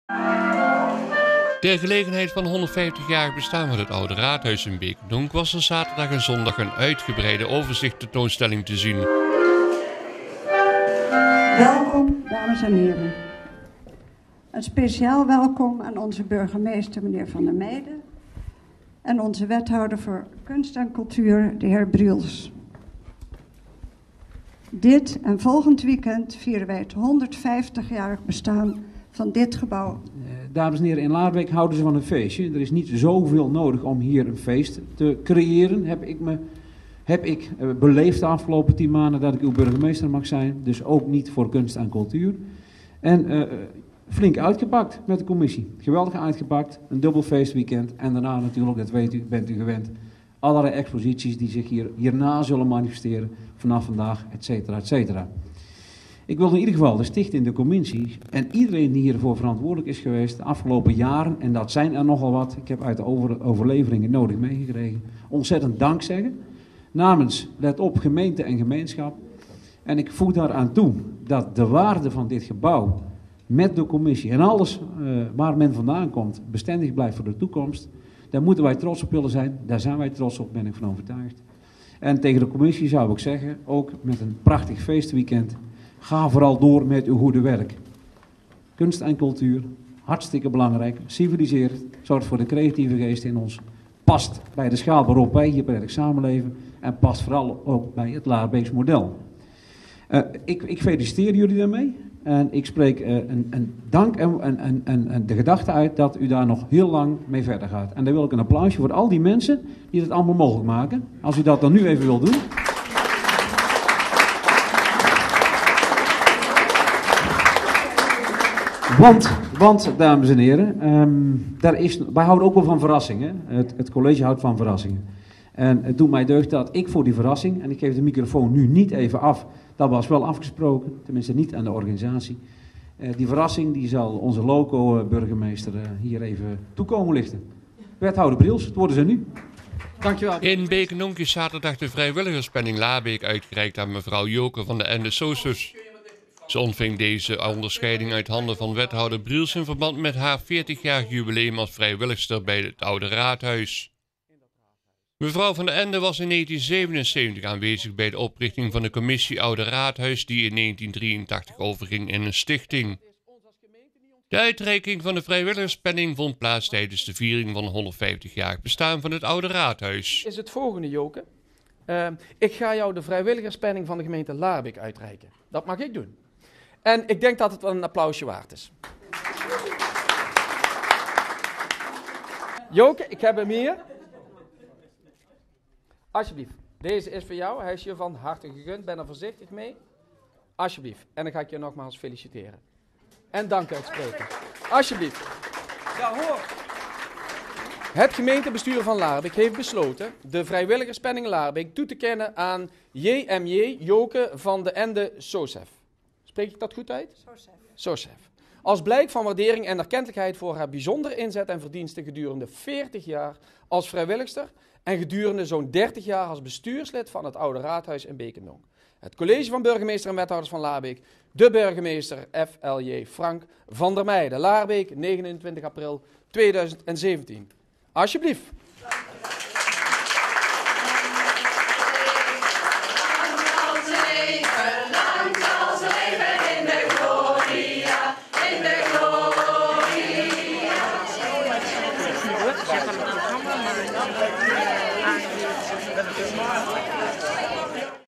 2017 04 29 BD 150 Jaar Oude Raadhuis en Vrijwilligerspenning
Beek & Donk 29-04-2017 Overzichtstentoonstelling 't Oude Raadhuis

Ter gelegenheid van het 150-jarig bestaan van 't Oude Raadhuis in Beek en Donk was er zaterdag en zondag een uitgebreide overzichtstentoonstelling te zien.

Vrijwilligerspenning Joke van den Ende

In Beek en Donk is zaterdag de Vrijwilligerspenning Laarbeek uitgereikt aan mevrouw Joke van den Ende-Sosef. Zij ontving deze onderscheiding uit handen van wethouder Briels in verband met haar 40-jarig jubileum als vrijwilligster bij 't Oude Raadhuis. Mevrouw Van den Ende was in 1977 aanwezig bij de oprichting van de commissie Oude Raadhuis die in 1983 overging in een stichting. De uitreiking van de Vrijwilligerspenning vond plaats tijdens de viering van het 150-jarig bestaan van ’t Oude Raadhuis.